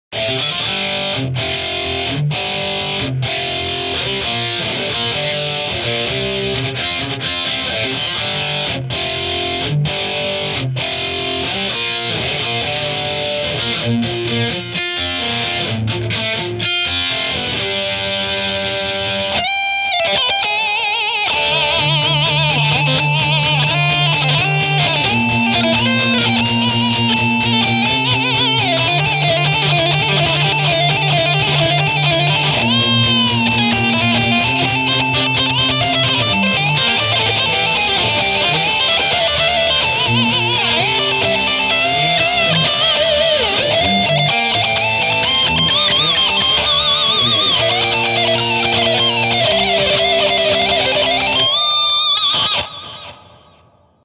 ROCKMAN XPR + EQ + 自作IR(Vintage30 x MD421-U4)テス
画像にあるサスティナーは使ってません。